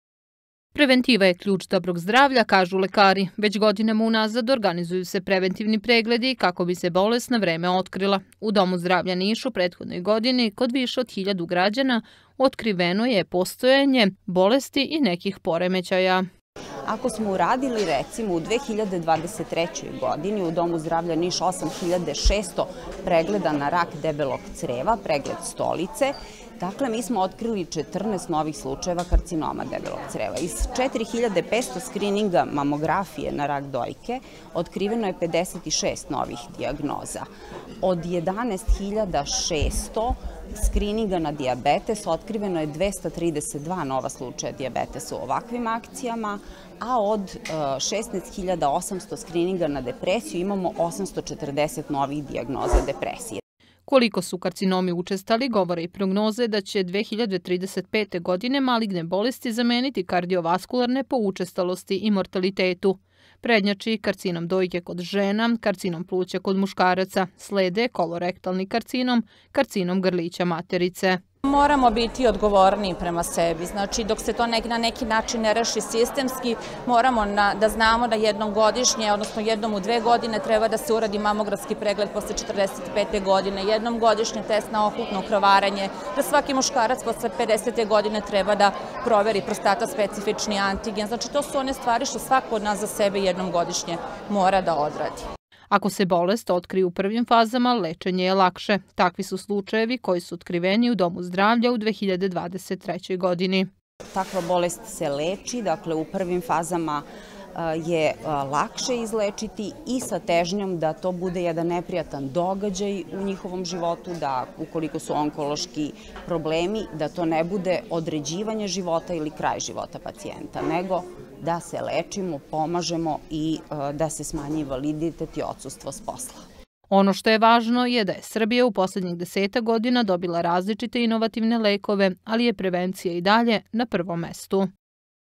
ŠTA SU OTKRILI PREVENTIVNI PREGLEDI?
Niški Dom zdravlja uradio je u prethodnoj godini hiljade skrininga na različite bolesti. U tom periodu otkriveno je 14 karcinoma debelog creva, kod 56 žena otkriven je rak dojke, 232 nova slučaja dijabetesa i 840 dijagnoza depresije. Lekari podsećaju na značaj preventivnih pregleda: Što se bolest ranije otkrije to je procenat izlečenja veći.